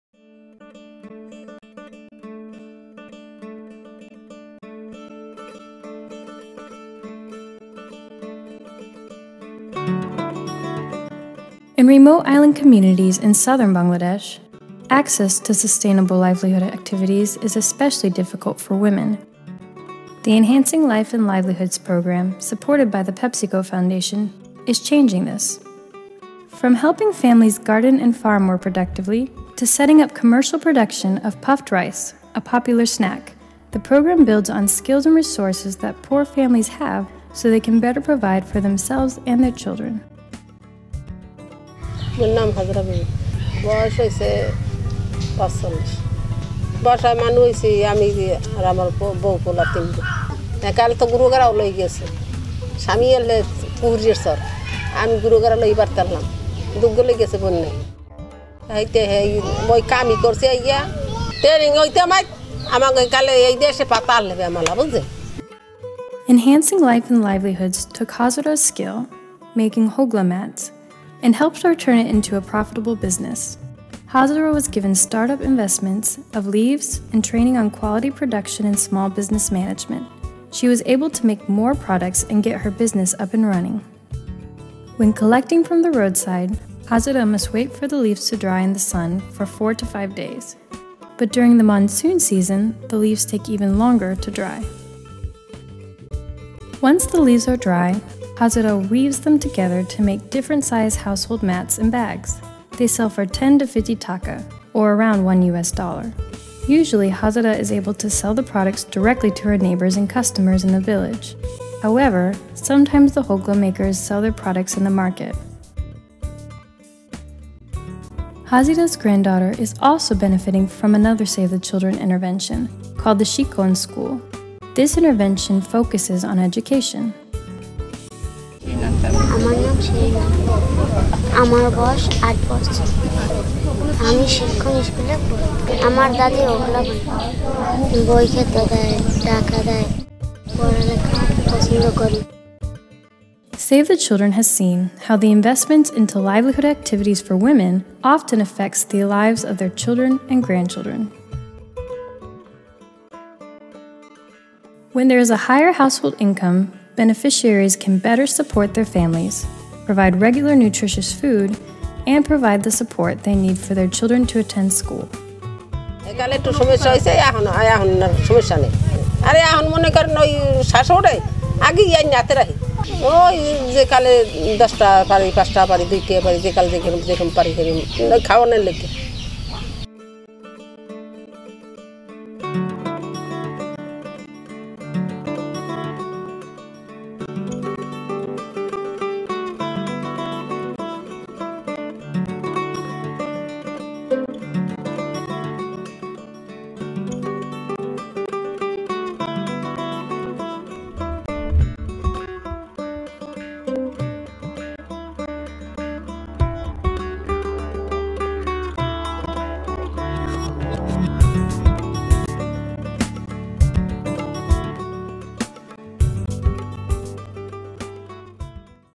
Bangladesh: Creating Opportunities for Women
Meet Hezara Begum, a 45 year old widow living in a remote island community in Bangladesh. Through a Pepsico supported program, Enhancing Life and Livelihoods, Save the Children helped her to use her existing skills and resources to build a small, commercially viable business producing and selling hogla mats. Now, like many other families who benefit from the Enhancing Life and Livelihoods program, Hezara can afford to send her granddaughter to school.